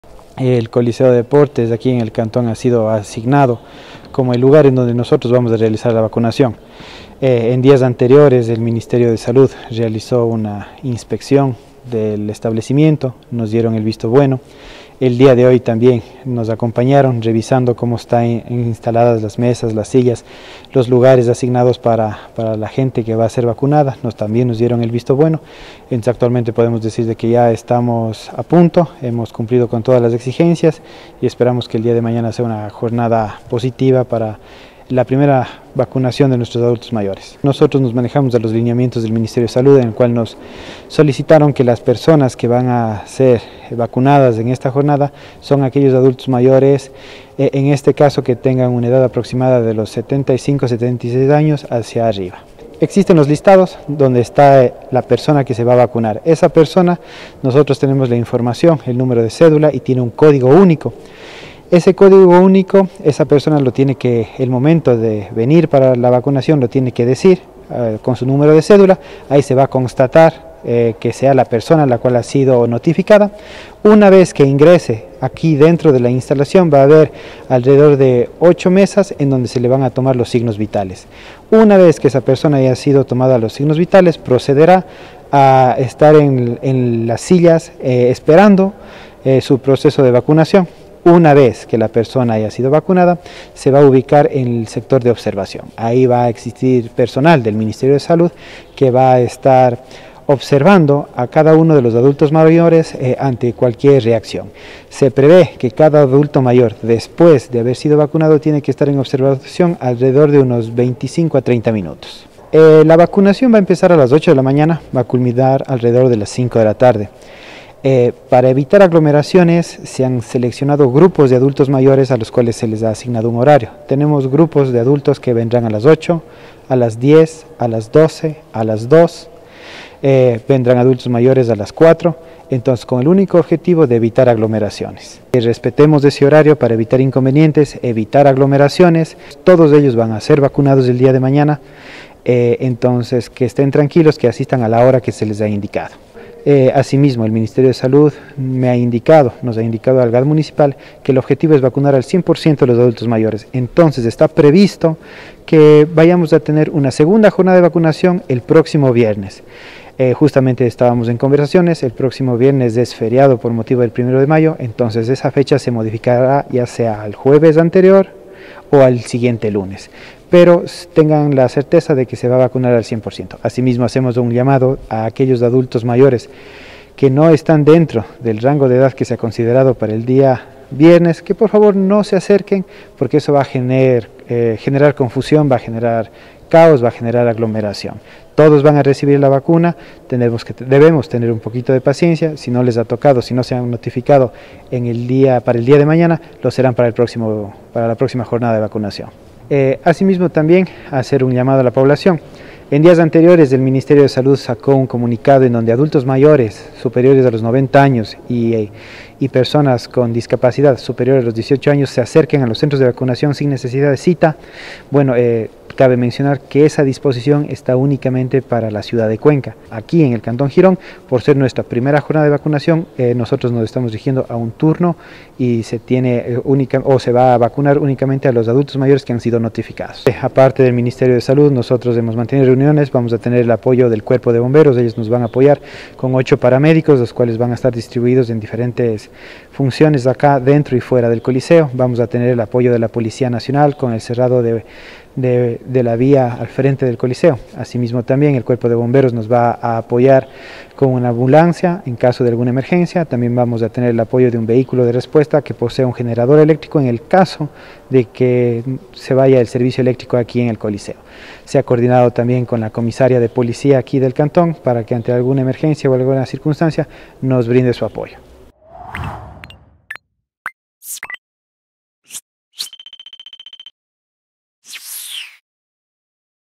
464 vacunas se aplicarán a adultos mayores en Girón
Personas adultas mayores, cuya edad sobrepasa los 72 años, que han sido registradas en Girón y recibieron la notificación y código, serán vacunadas este día viernes contra la covid-19 en el coliseo de deportes Esteban Lucero. El local cuenta con el visto bueno del Ministerio de Salud, el listado de los beneficiarios también fue elaborado en base a los lineamientos requeridos indicó el secretario del COE cantonal y director de Gestión de Riesgos del GAD Municipal de Girón. Se pide que únicamente acudan las personas notificadas y en la hora indicada para evitar contratiempos. Para la próxima semana se tiene previsto la llegada de un nuevo lote de vacunas para el resto de personas adultas mayores que en esta ocasión no fueron llamadas.